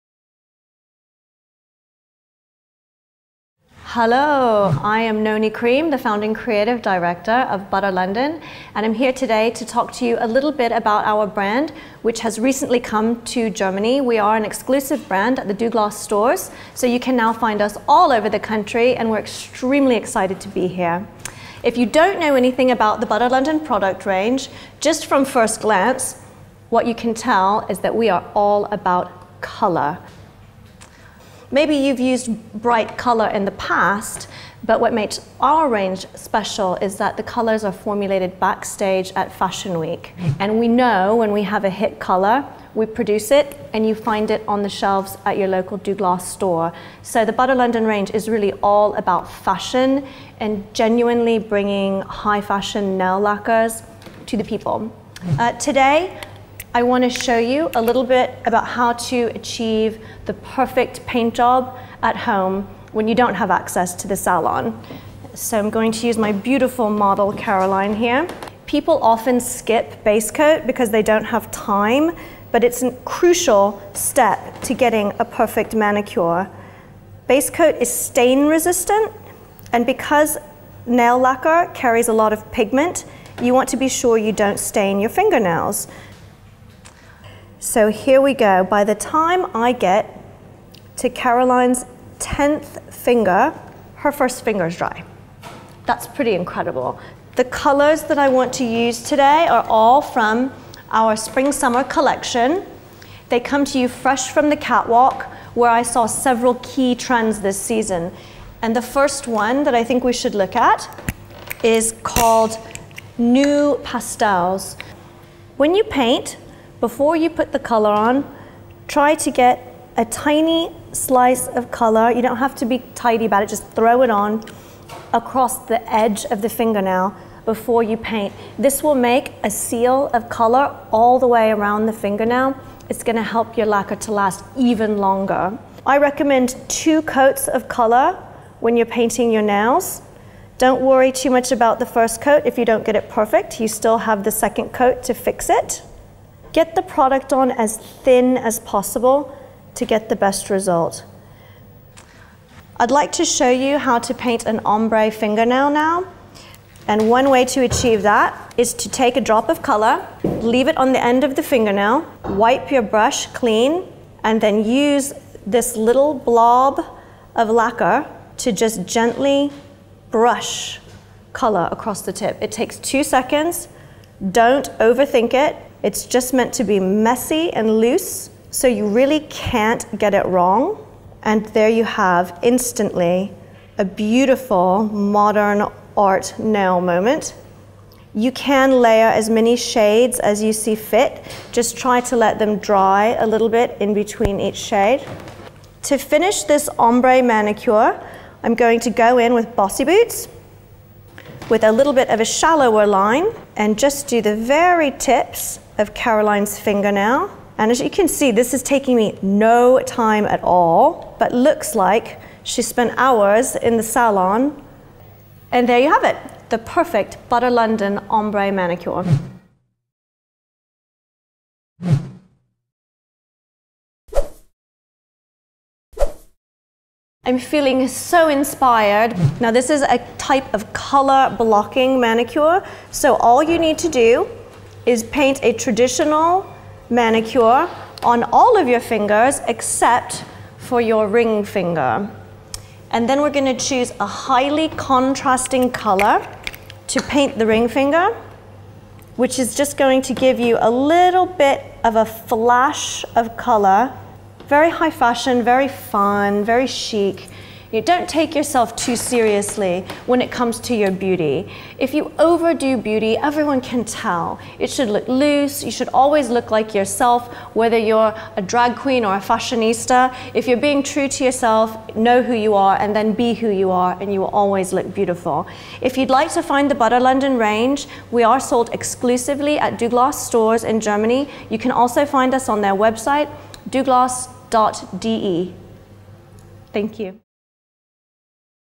butter LONDON "Spring/Summer Collection" - Nail Tutorial mit Nonie Creme
butter LONDONS Gründerin Nonie Creme zeigt Euch Ihre Nagellack-Trend Looks mit der "Spring/Summer Collection 2012" und gibt Euch Tipps für ein langanhaltendes und aufregendes Nagel-Make-up!